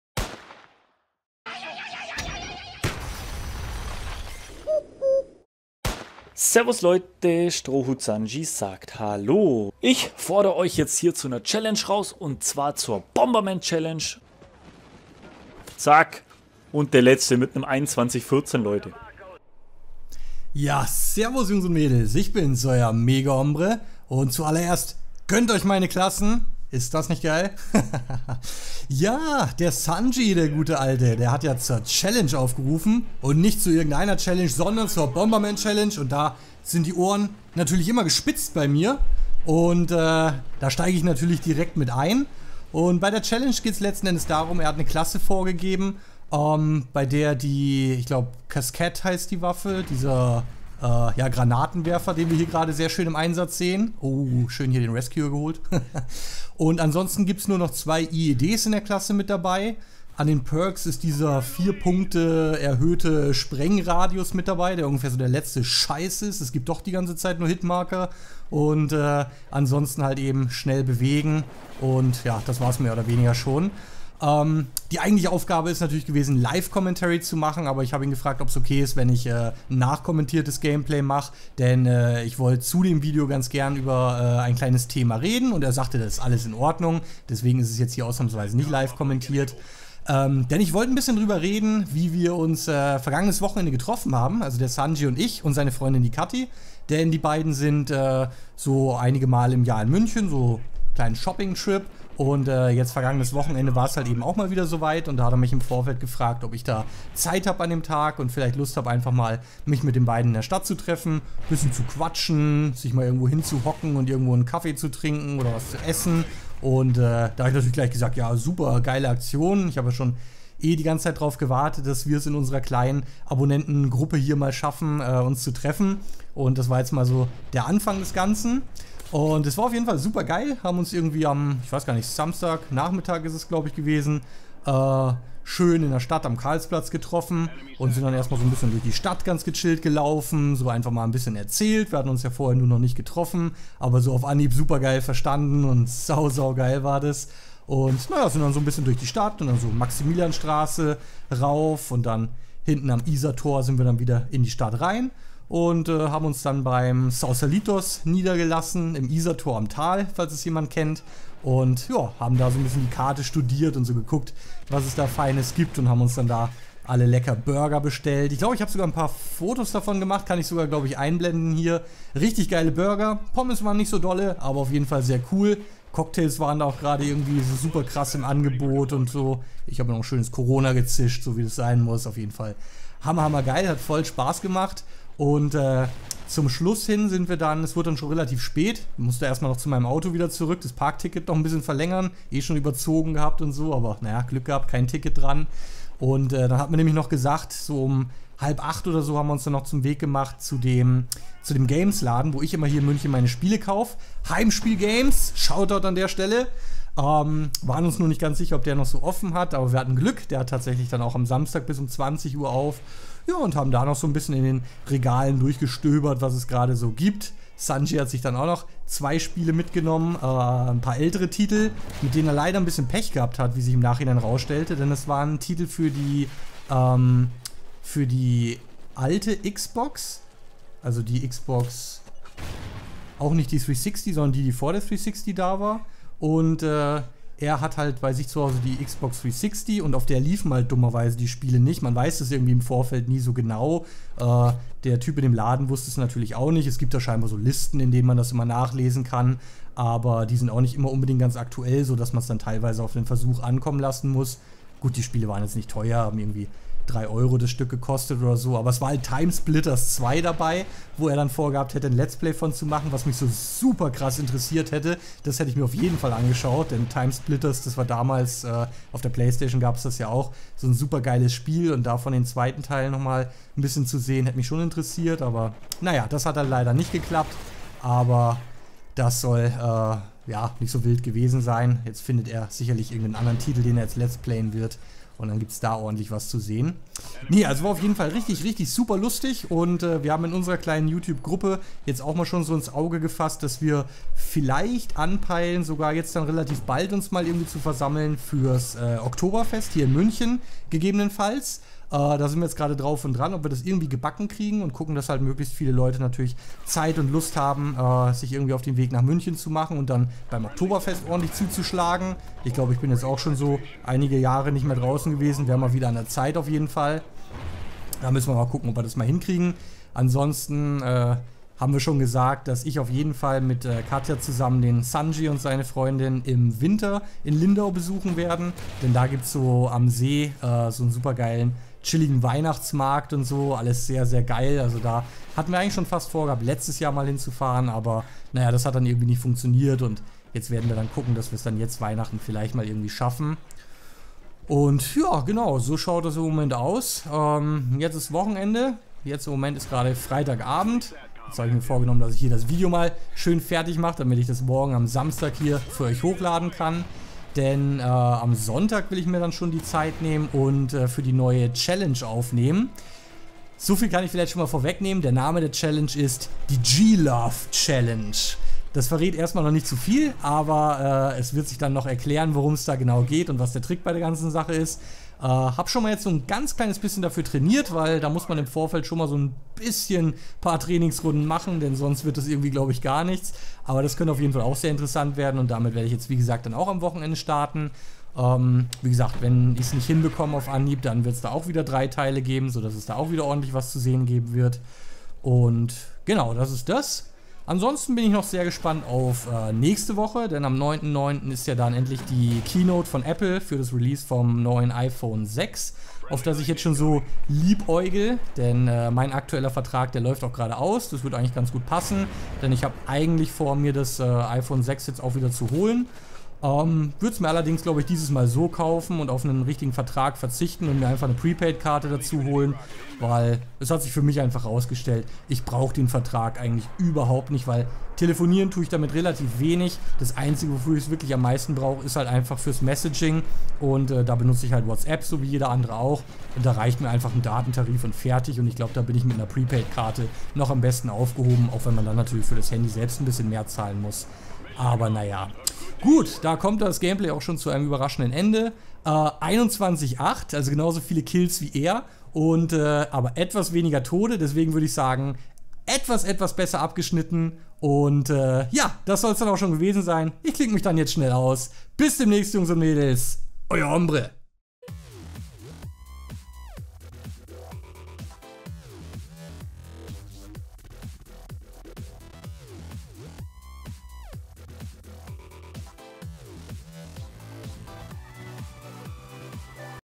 Strohhut Sanji - Bomberman Challenge - Wer schafft mehr Kills?
Servus Jungs und Mädels,

der Sanji hat zu einer neuen Variante der Bomberman Challenge aufgerufen und da bin ich natürlich dabei.

Es geht darum, mit der vorgegebenen Klasse so viele Kills wie möglich in einem öffentlichen Team Deatchmatch zu sammeln.

Er hat mit einem Spiel 21-14 vorgelegt, mal sehen, was ich so reissen kann :)

Viel Spass und bis bald wieder,
Euer Hombre